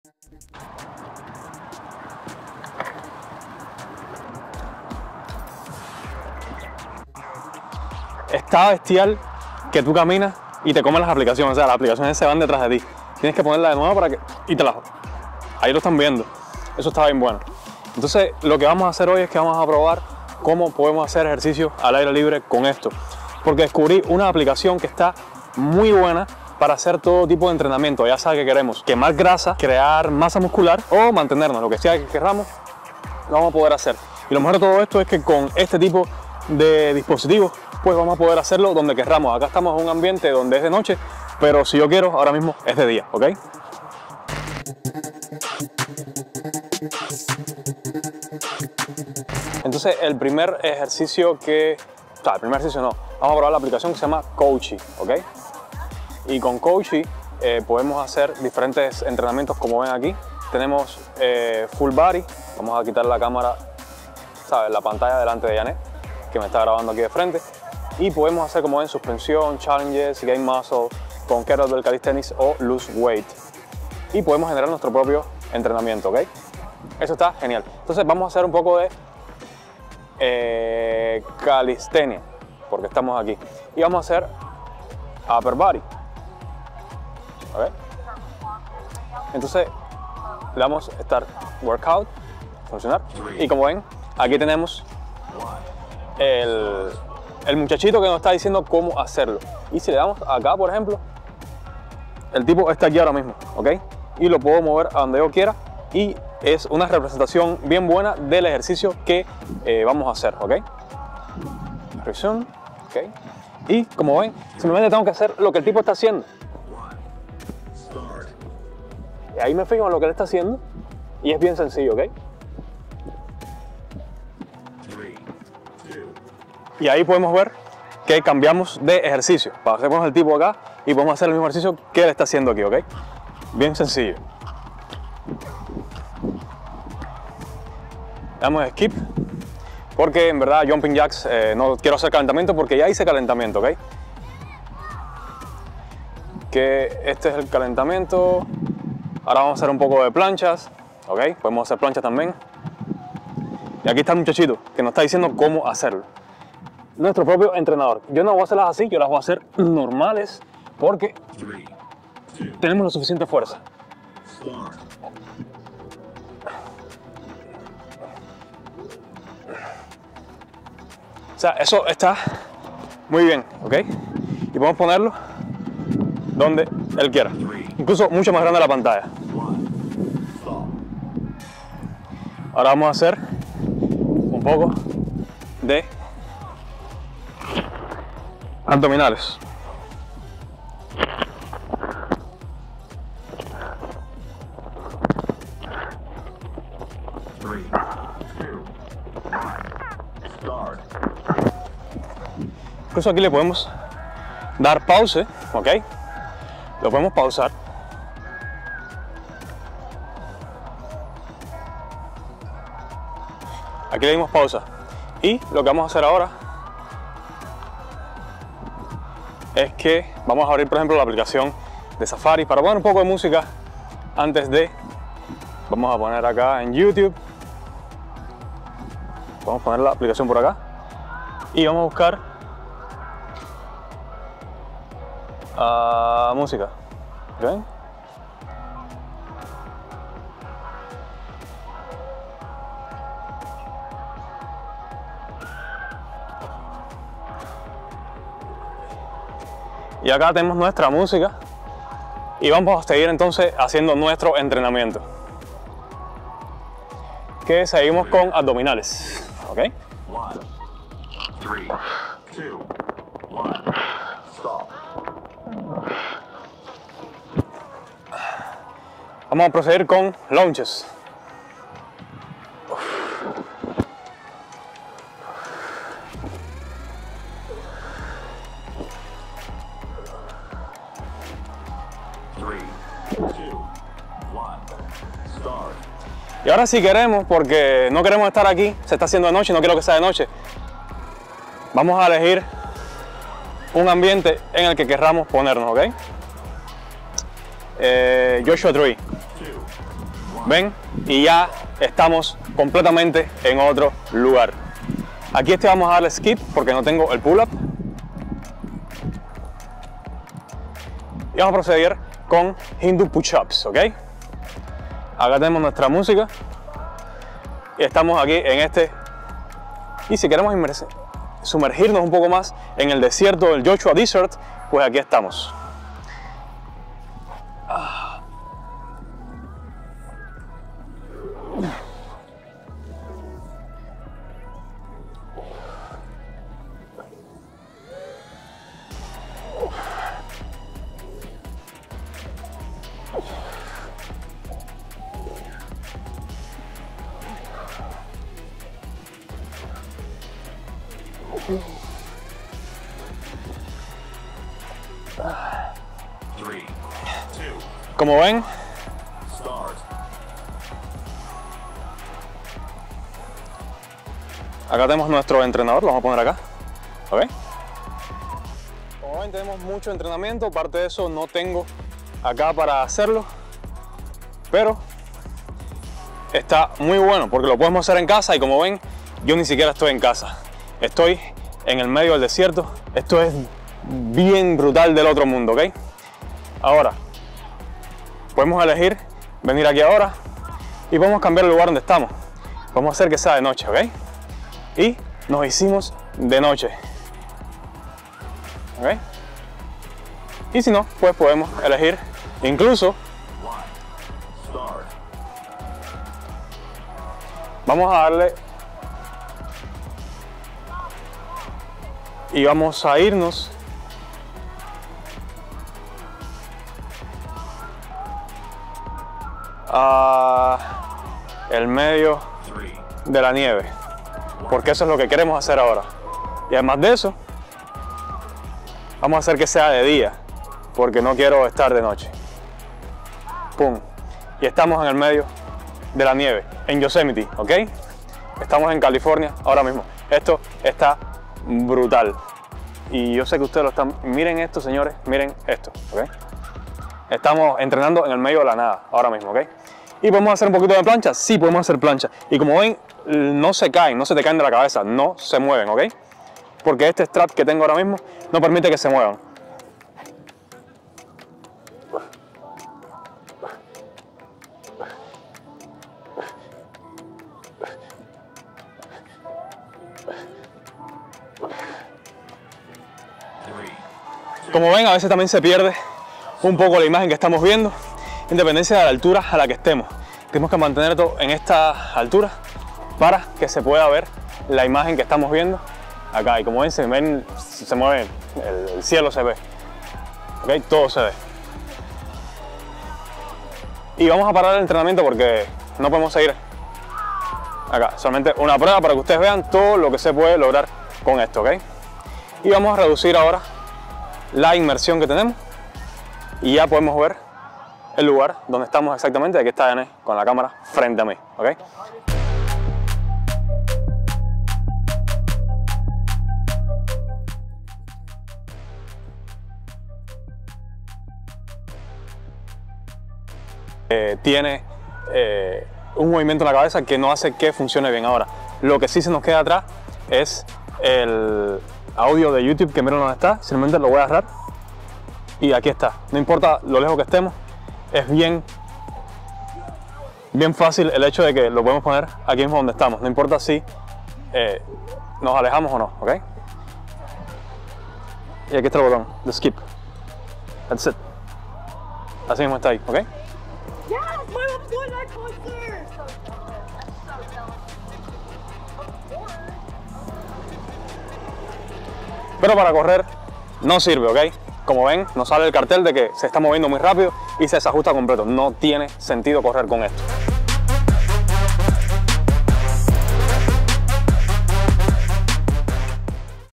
sobreviví un entrenamiento FITNESS con el Apple Vision Pro
Vale la pena pagar 3500 USD para ponernos fitness?? Estos son los Apple Vision Pro y asi los uso para mis entrenamientos 🔥😅